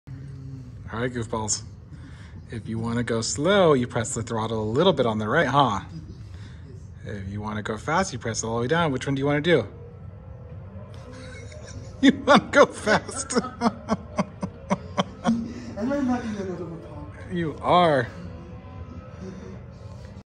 Spencer first driving experience and his contagious laughter.
Its been a while so hello world of youtube! You might remember Spencer, he has autism and blindness but still enjoys everything going FAST!

I recently put together a racing simulator for us and couldnt wait to get Spencer on it, hope you enjoy!